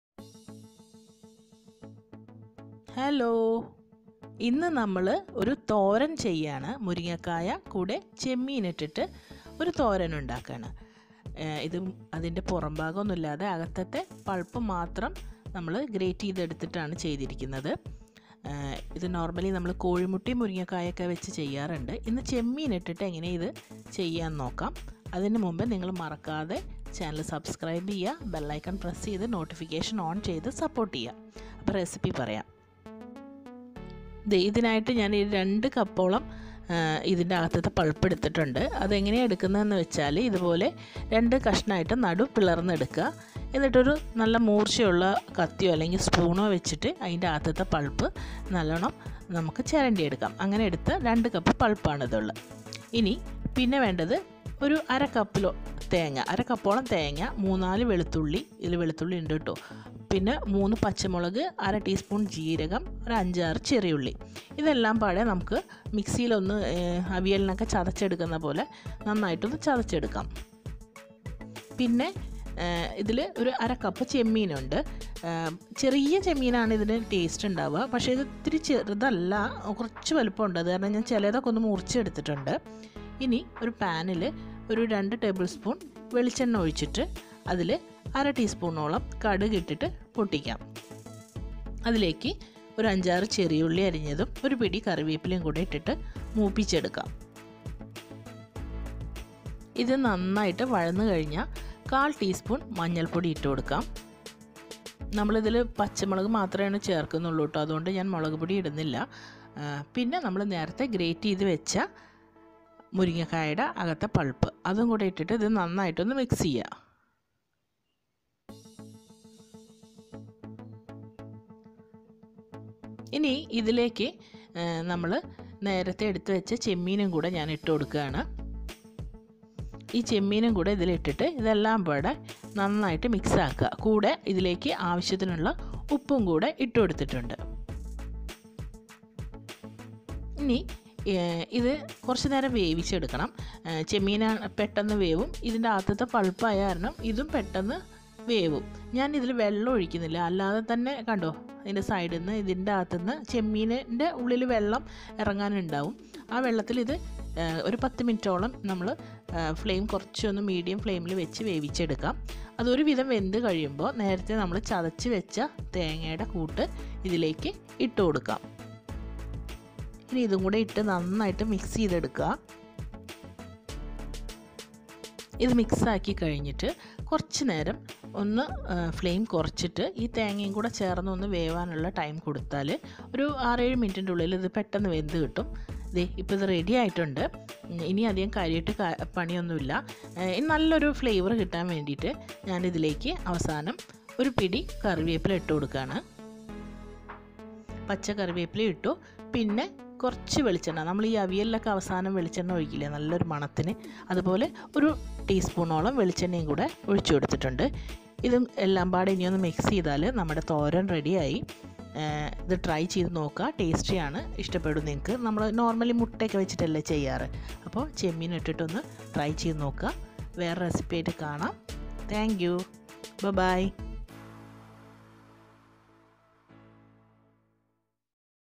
മുരിങ്ങക്കായ ചെമ്മീൻ തോരൻ / തോരൻ / muringakka chemmeen thoran / drumstick recipe / Thoran / chemmeen
മുരിങ്ങക്കായയും ചെമ്മീനും ഒരുമിച്ച് ചേർത്തൊരു തോരൻ ഉണ്ടാക്കി നോക്കാം നമുക്ക്

മുരിങ്ങക്ക ചെമ്മീൻ തോരൻ

Music